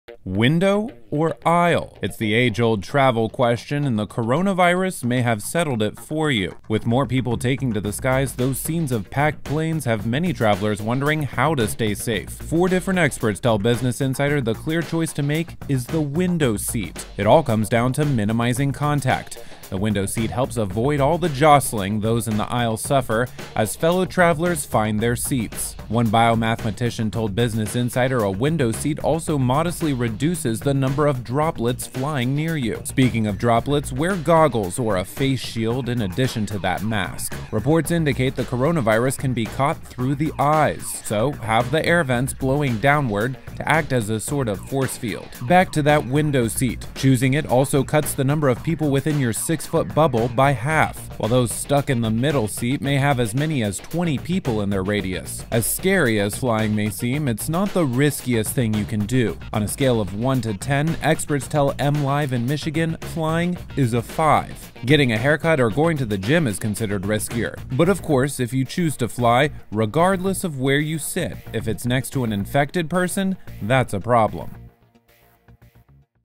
Window or Aisle? The Clear Answer When Flying During Coronavirus
Window or aisle? It’s the age-old travel question, and the Coronavirus may have settled it for you.